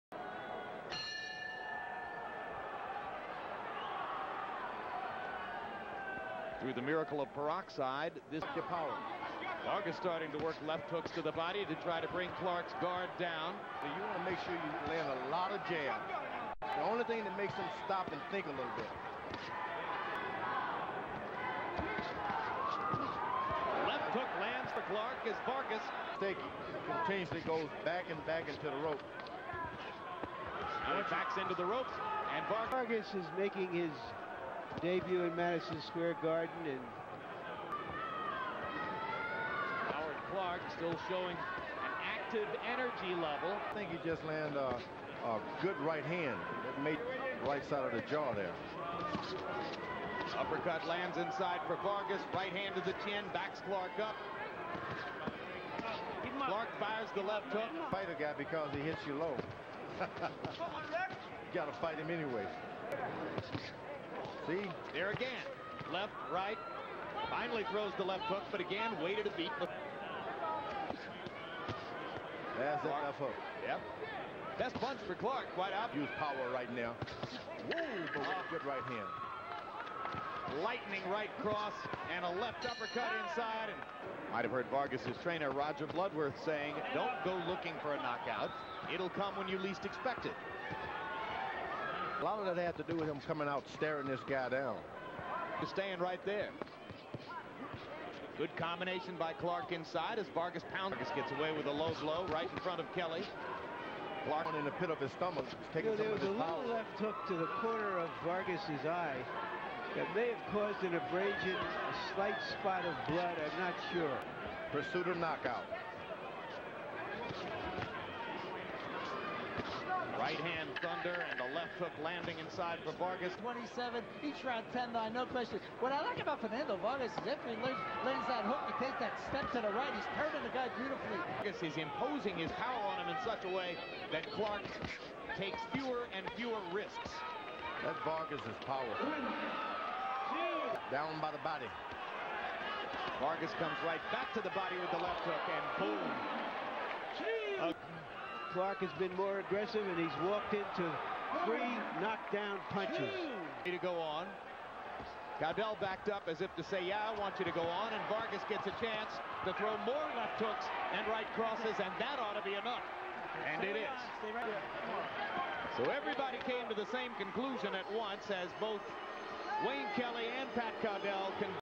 Fernando Vargas Vs Howard Clarke Highlights
"Ferocious" Fernando Vargas Vs Howard Clarke. Por el Titulo Mundial IBF de Peso Superwelter. Vargas hace la 1era. Defensa de su Titulo.